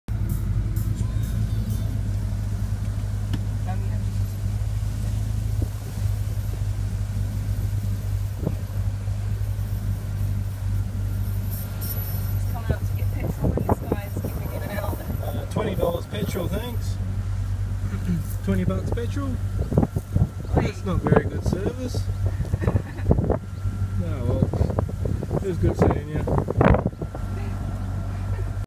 Interesting Service at Exmouth Service station
Drove up the the bowser and i asked for $20 petrol. Service attendant just got up and walked off. How rude!